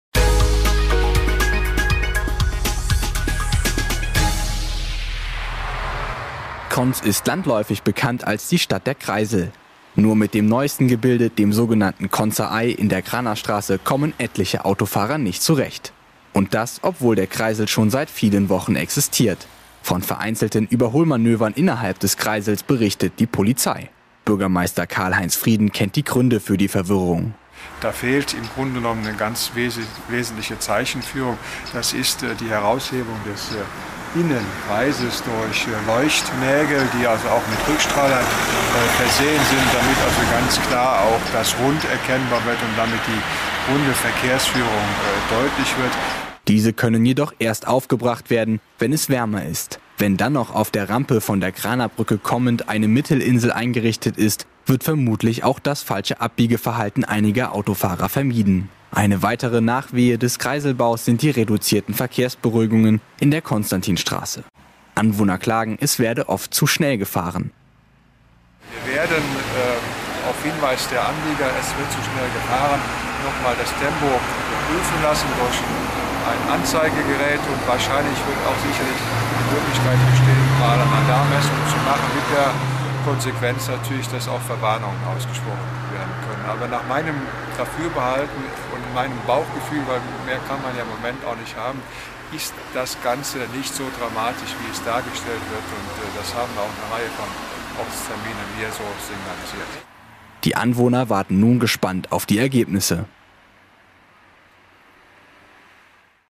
volksfreundtv Verwirrung am Konzer Mini Kreisel
Am neuen Konzer Mini-Kreisel wird ziemlich wirr gefahren. Der Grund: Die Markierung der Innen-Insel fehlt noch. Außerdem rollen die Autos von der Rampe komme...

volksfreundtv:, Verwirrung, am, Konzer, MiniKreisel, volksfreundtv, Verwirrung, am, Konzer, Mini, Kreisel